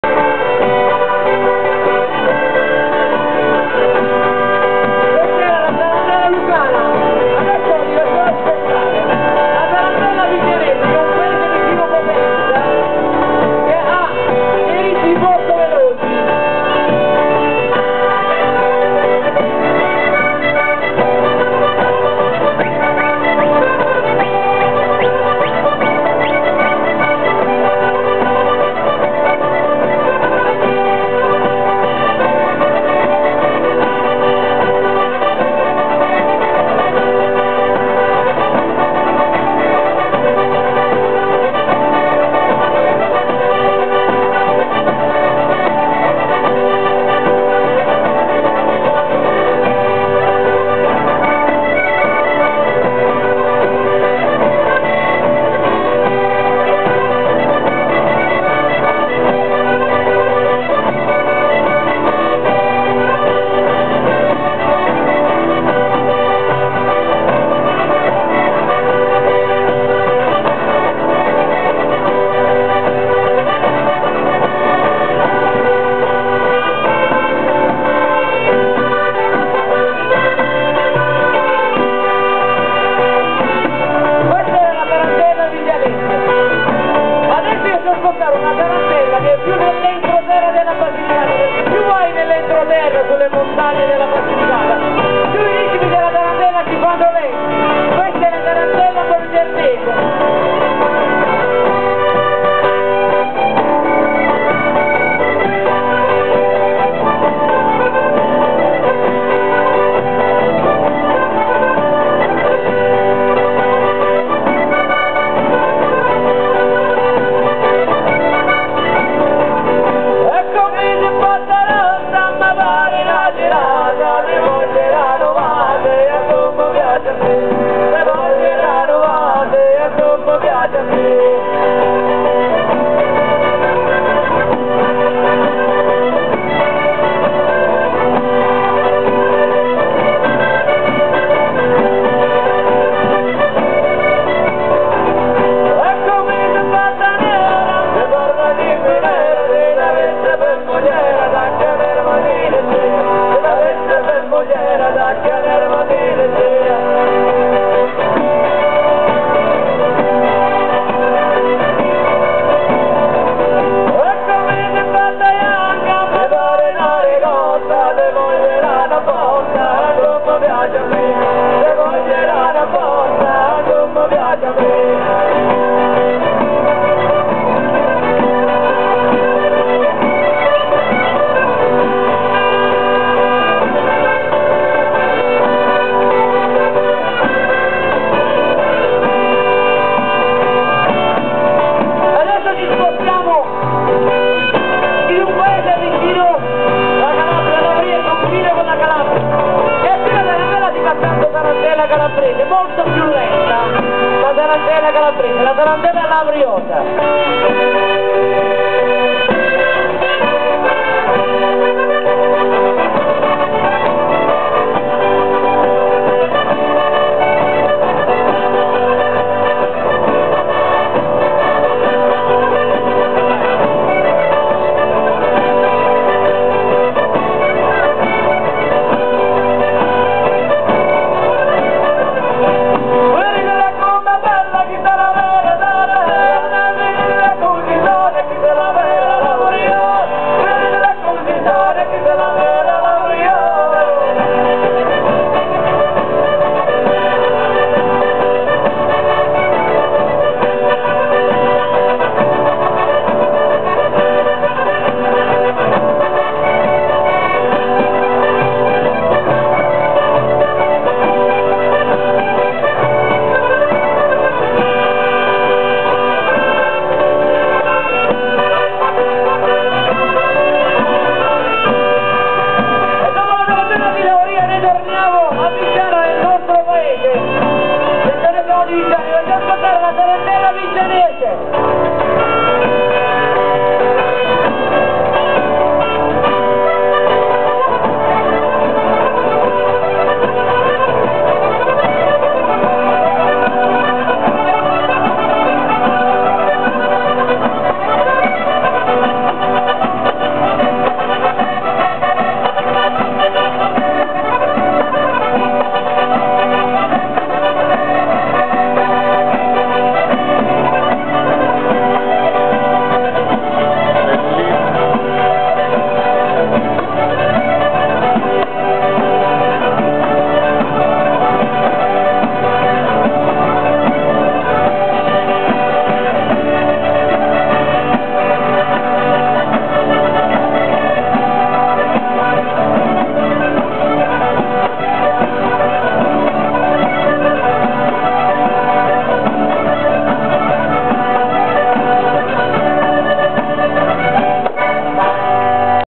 Amarimai alla juta a Montevergine tarantelle lucane
12 settembre 2011, Ospedaletto Alpinolo, la juta a Montevergine, suonano gli Amarimai, gruppo musicale di Viggiano.